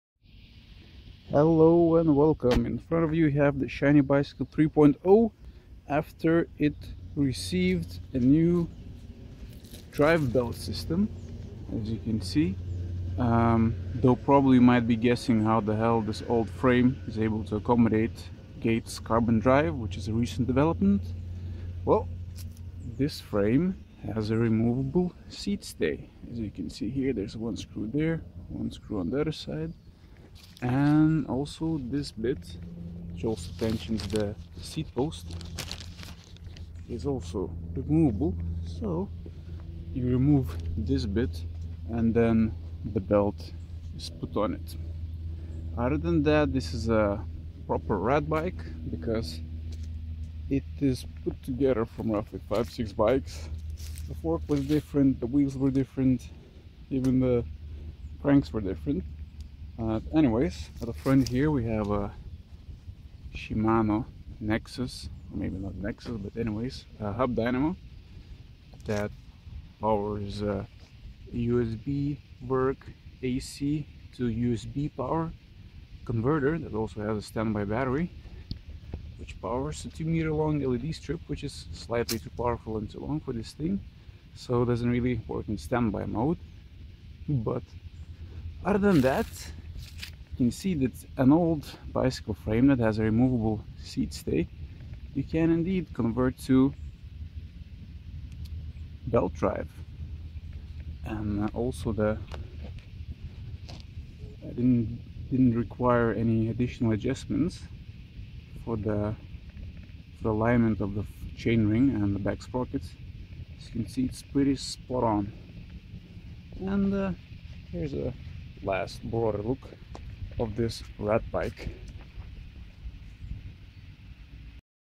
Shiny Bicycle (Lichtfiets) 3.0 Gates Carbon Drive upgrade
Upgraded an old Batavus Bicycle frame (Shiny Bicycle (Lichtfiets) 3.0 (a.k.a. RatCycle)), which has removable seat-stay and semi-horizontal drop-outs to Gates Carbon Drive belt drive-train system.

Hub Dynamo, USB-Werk and LED strip in Action at night (Bike was with a chain in the video)